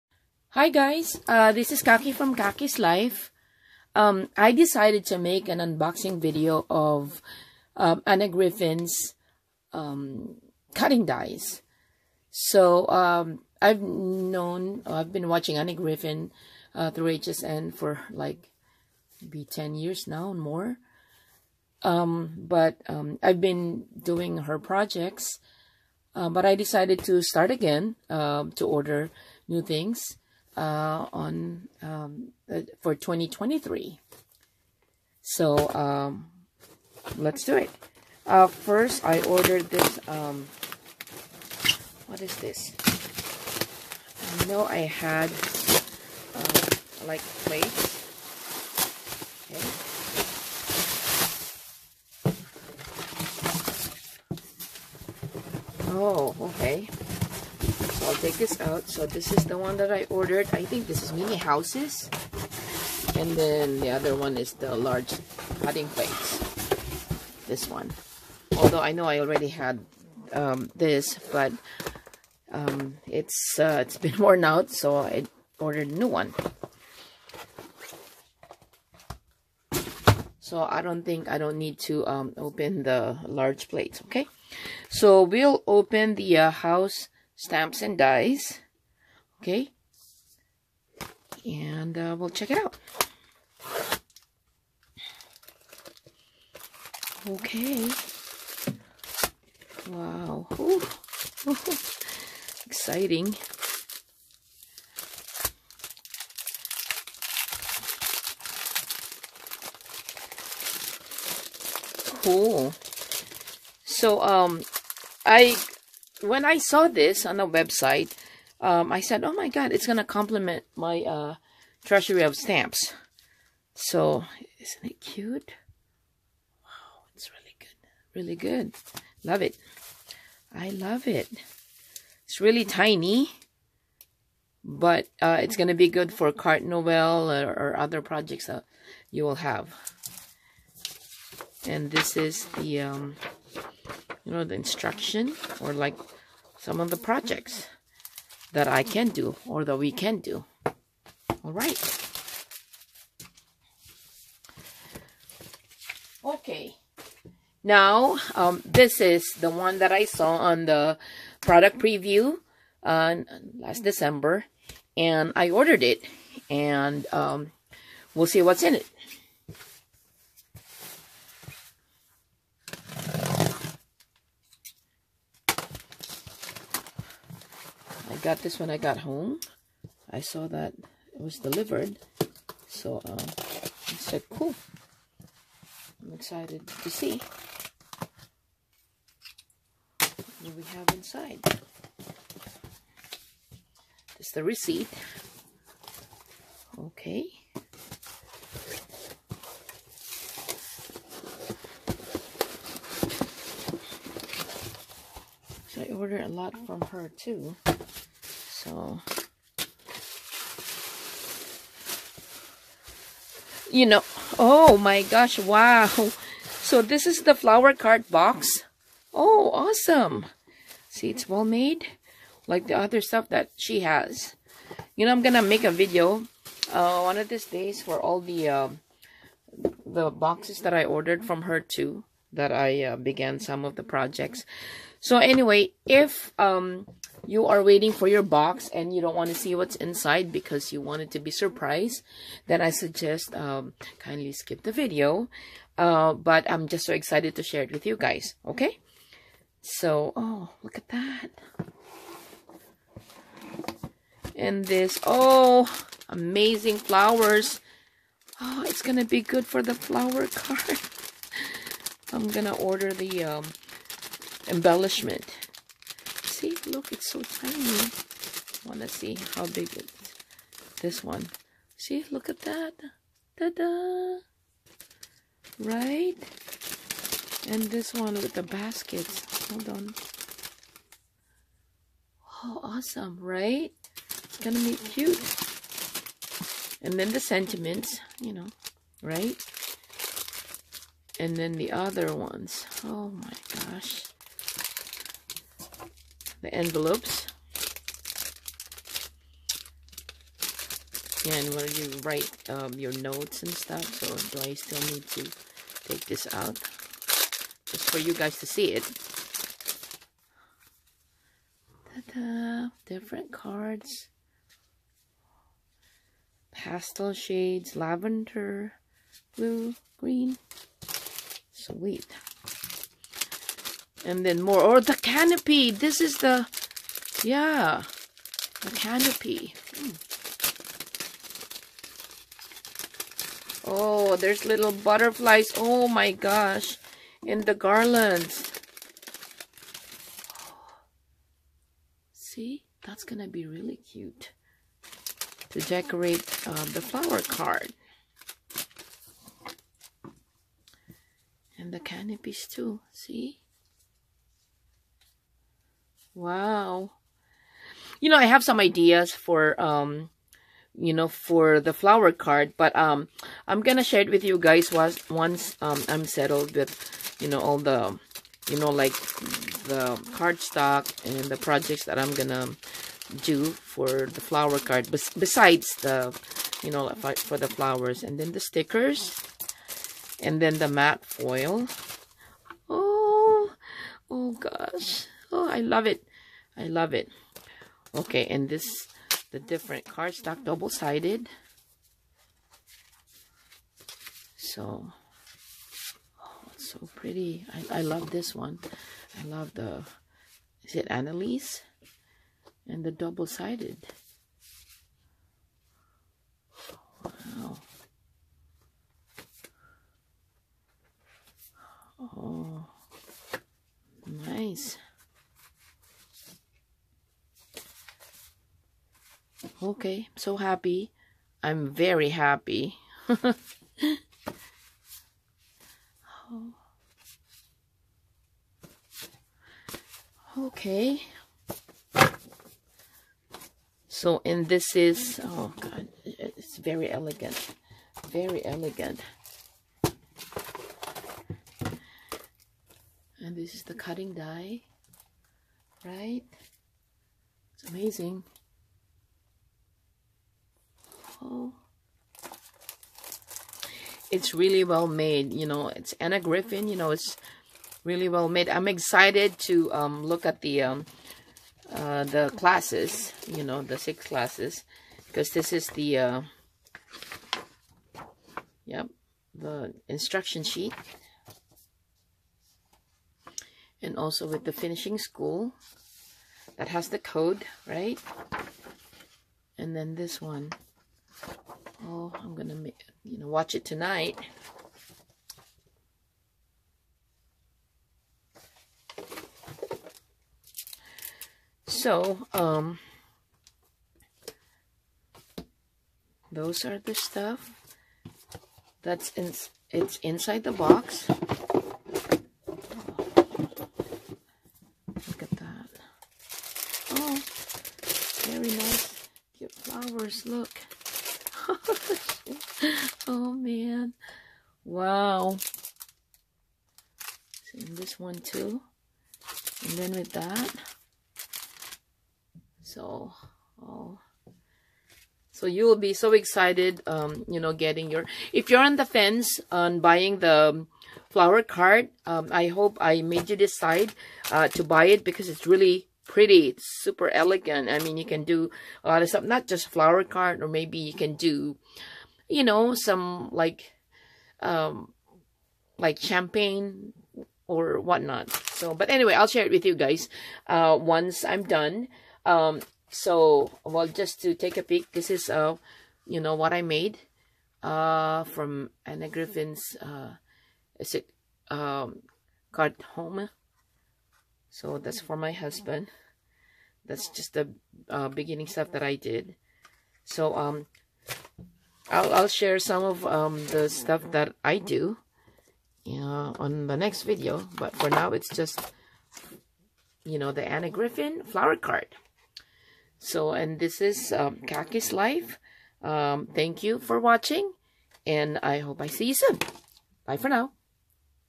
Anna Griffin's Flower Cart Craft Box: Unboxing video & House Stamps & Dies
This is the set of the Auto ship. Flower Cart .